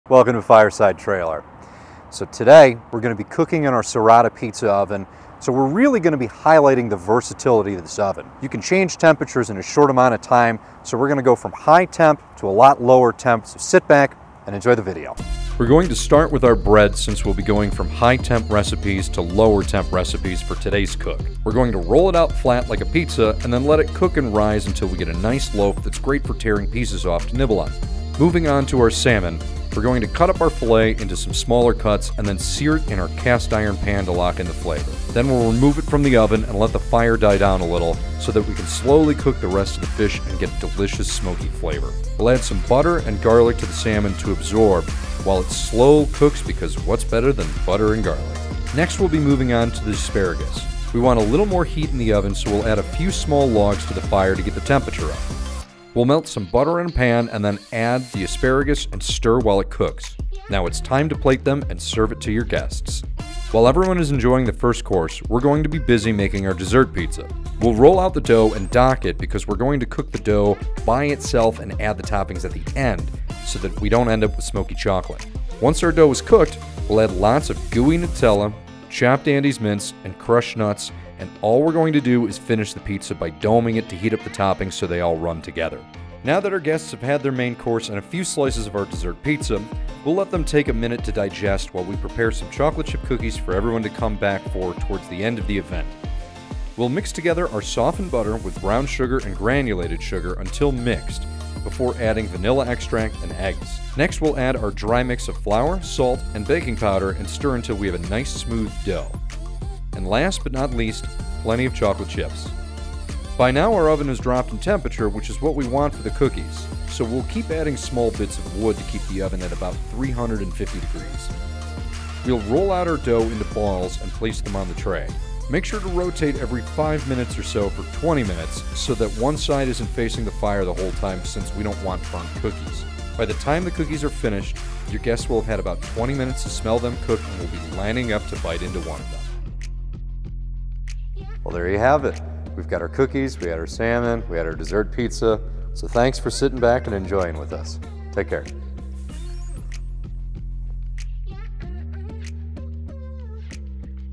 Salmon, asparagus, pita, dessert pizza, cookies
0:16 Pita bread
0:28 Salmon and asparagus
1:07 Dessert pizza
1:29 Cookies